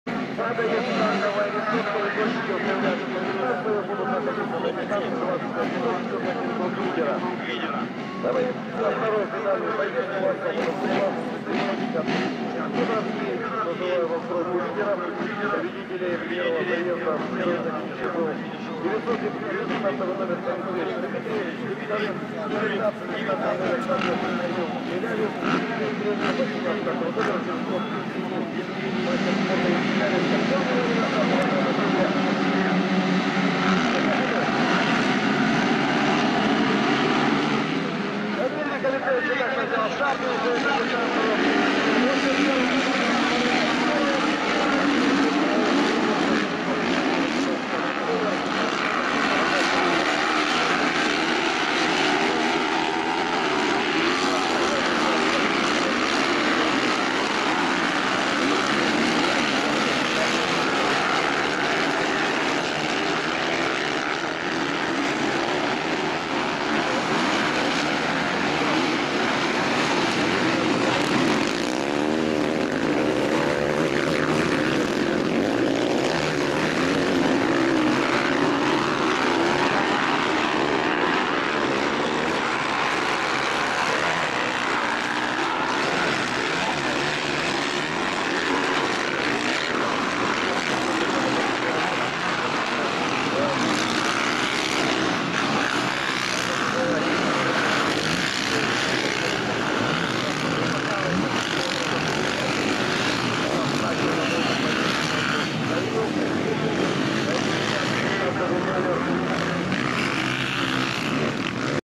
02.06.2012 2этап УрФО по мотокроссу.250 2 заезд.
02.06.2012
Мотокросс
Каменск-Уральский